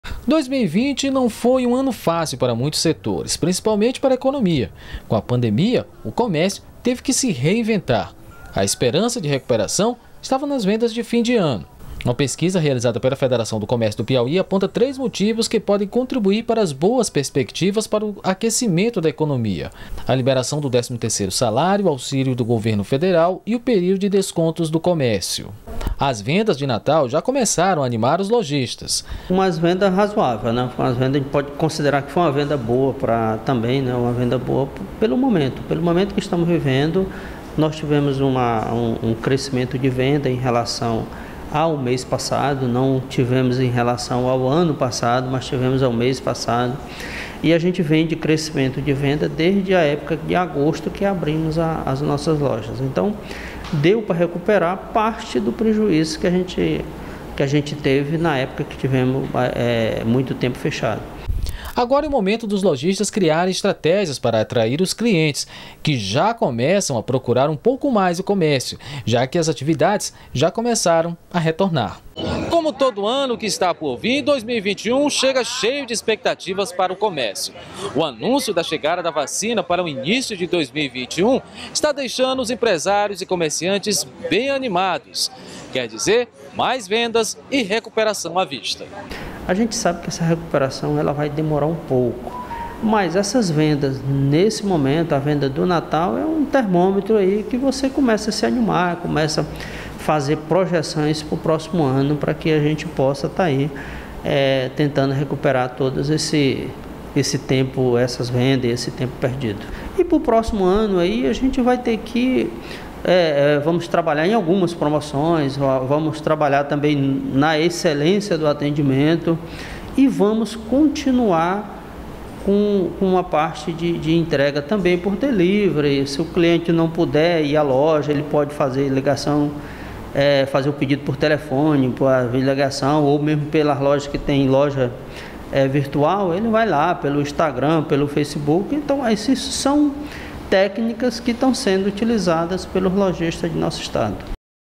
Comércio tem boa expectativa de venda para festas de fim de ano
A pandemia provocada pelo novo coronavírus trouxe o fechamento repentino das lojas físicas. Agora, com as festividades de fim de ano, são grandes as expectativas para o comércio, que tenta se recuperar da crise. A reportagem é de James Almeida e Roniel Matos, da TV Assembleia.